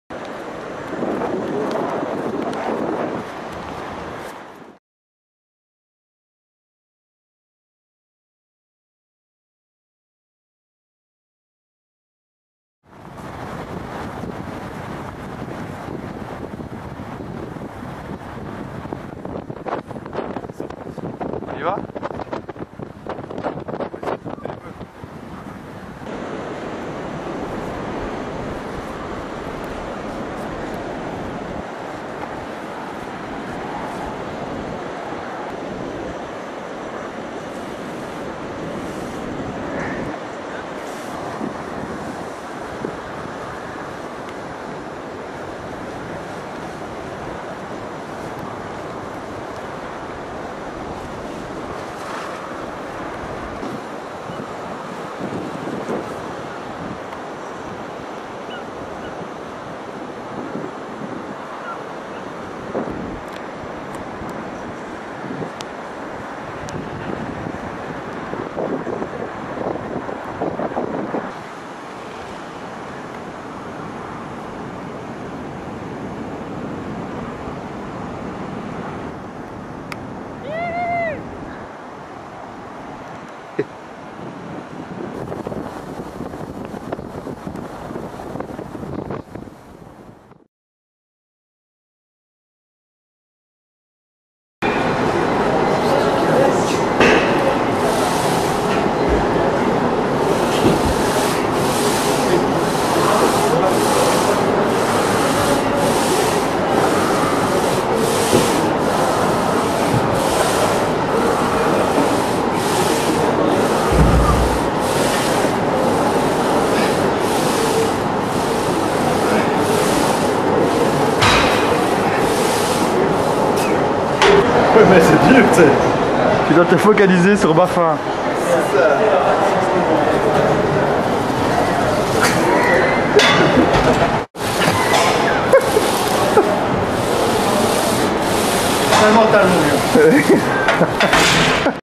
Wide Arctic: entraînement
Entraînement au Kite à Zeebruge et fitness chez David Lloyd